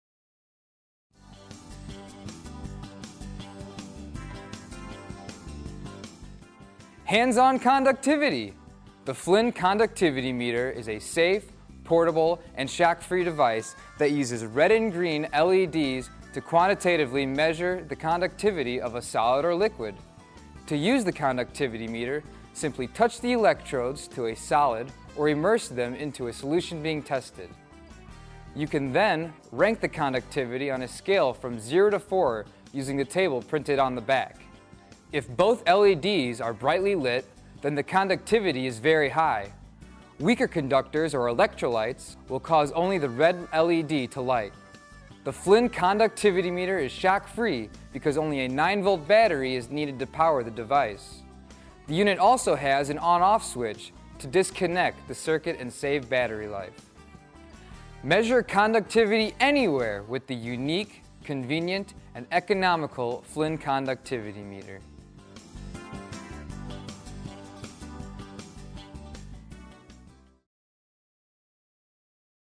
Flinn Conductivity Meter
Watch as the Flinn Scientific Staff demonstrates the ""Flinn Conductivity Meter.""